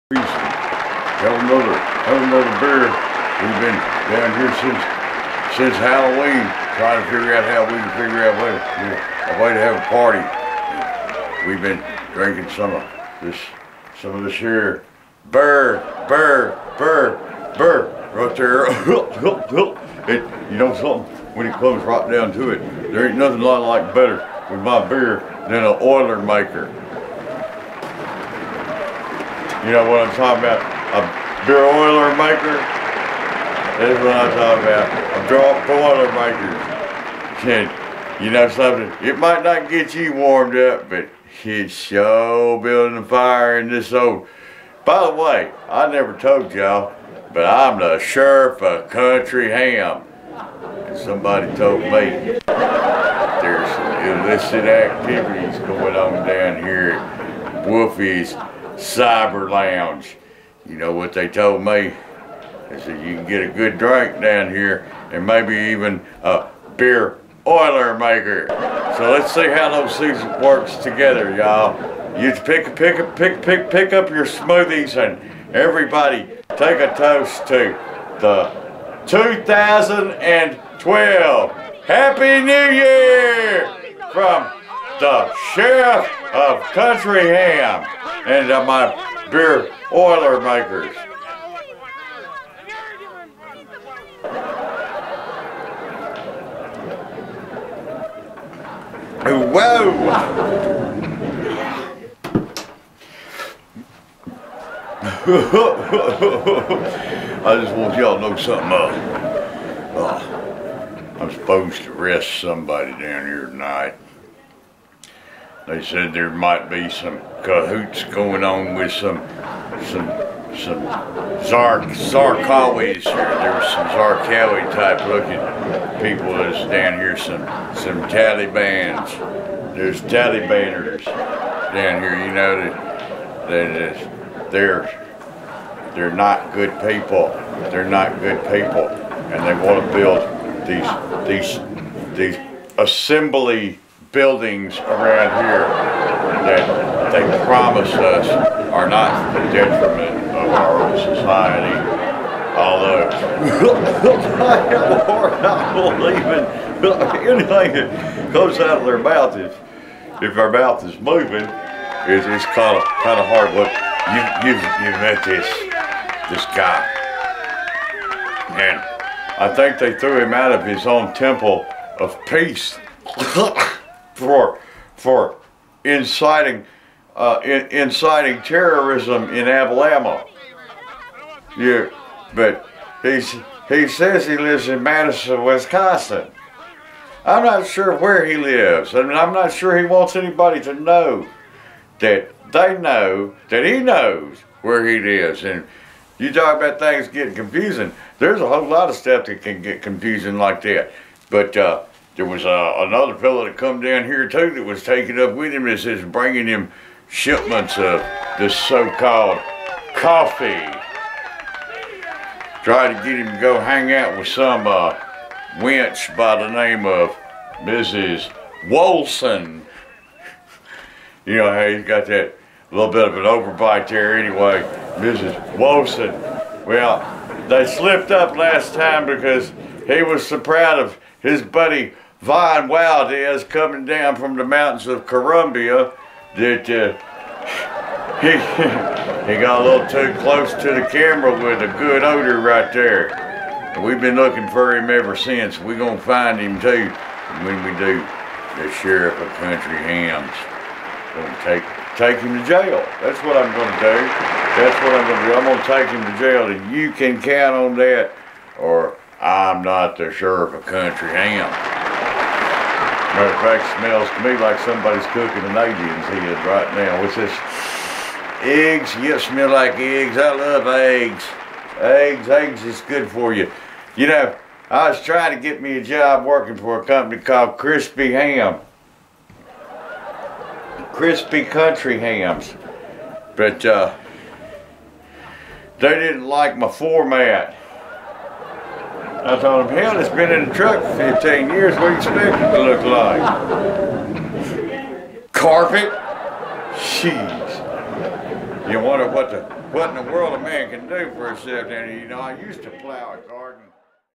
Sheriff of Country Hamm
Comedy video! Wolfy is the Sheriff of Country Hamm